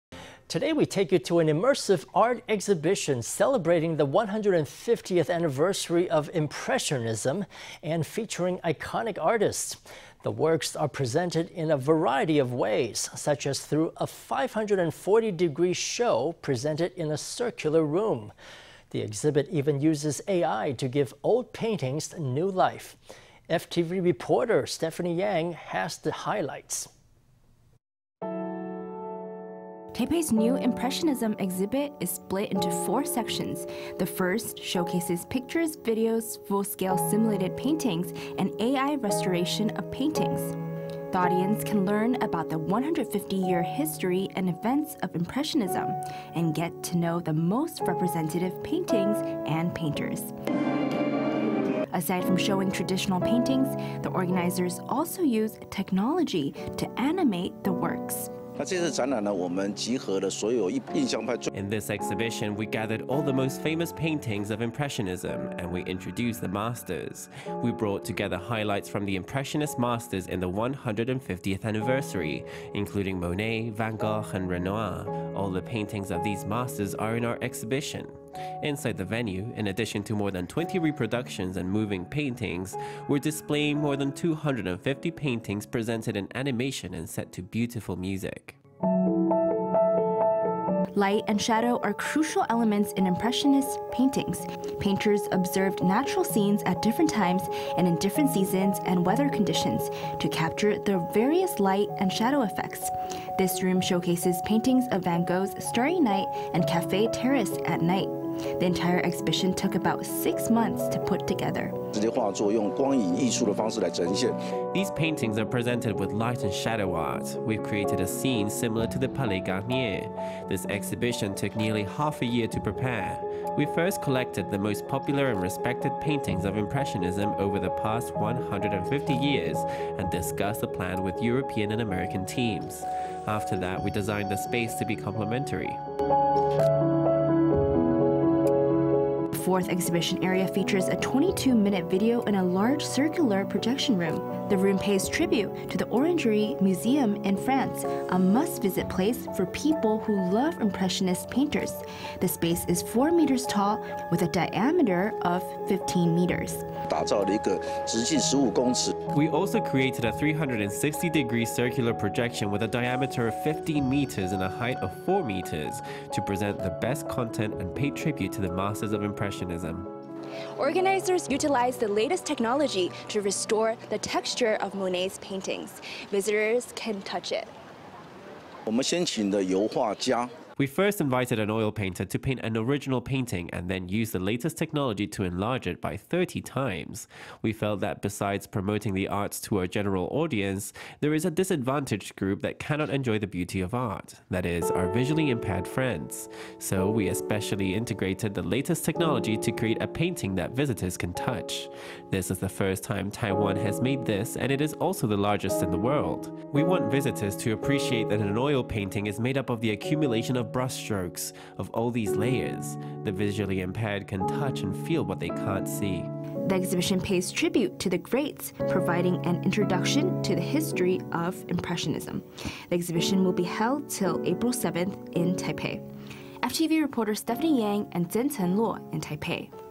Taipei exhibit celebrates 150th anniversary of impressionism｜Taiwan News
Today we take you to an immersive art exhibition celebrating the 150th anniversary of impressionism, featuring iconic artists. The works are presented in a variety of ways, such as through a 540-degree show presented in a circular room. The exhibit even uses AI to give old paintings new life. FTV reporter Stephany Yang has the highlights.

Taipei’s new impressionism exhibit is split into four sections. The first showcases pictures, videos, full-scale simulated paintings, and AI restoration of paintings. The audience can learn about the 150-year history and events of impressionism, and get to know the most representative paintings and painters. Aside from showing traditional paintings, the organizers also used technology to animate the works.

Garrick Cheng
General manager
In this exhibition, we gathered all the most famous paintings of impressionism, and we introduce the masters. We brought together highlights from the impressionist masters in the 150th anniversary, including Monet, Van Gogh, and Renoir. All the paintings of these masters are in our exhibition. Inside the venue, in addition to more than 20 reproductions and moving paintings, we’re displaying more than 250 paintings presented in animation and set to beautiful music.

Light and shadow are crucial elements in impressionist paintings. Painters observed natural scenes at different times, and in different seasons and weather conditions, to capture the various light and shadow effects. This room showcases paintings of Van Gogh’s "Starry Night" and "Cafe Terrace at Night." The entire exhibition took about six months to put together.

Garrick Cheng
General manager
These paintings are presented with light and shadow art. We created a scene similar to the Palais Garnier. This exhibition took nearly half a year to prepare. We first collected the most popular and respected paintings of impressionism over the past 150 years and discussed the plan with European and American teams. After that, we designed the space to be complementary.

The fourth exhibition area features a 22-minute video in a large circular projection room. The room pays tribute to the Orangery Museum in France, a must-visit place for people who love impressionist painters. The space is 4 meters tall, with a diameter of 15 meters.

Garrick Cheng
General manager
We also created a 360-degree circular projection with a diameter of 15 meters and a height of 4 meters, to present the best content and pay tribute to the masters of impressionism.

Stephany Yang
FTV reporter
Organizers used the latest technology to restore the texture of Monet’s painting. Visitors can touch it.

Garrick Cheng
General manager
We first invited an oil painter to paint an original painting and then used the latest technology to enlarge it by 30 times. We felt that besides promoting the arts to our general audience, there is a disadvantaged group that cannot enjoy the beauty of art – that is, our visually impaired friends. So we specially integrated the latest technology to create a painting that visitors can touch. This is the first time Taiwan has made this and it is also the largest in the world. We want visitors to appreciate that an oil painting is made up of the accumulation of brush strokes, of all these layers. The visually impaired can touch and feel what they can’t see.

The exhibition pays tribute to the greats, providing an introduction to the history of impressionism. The exhibition will be held till April 7 in Taipei.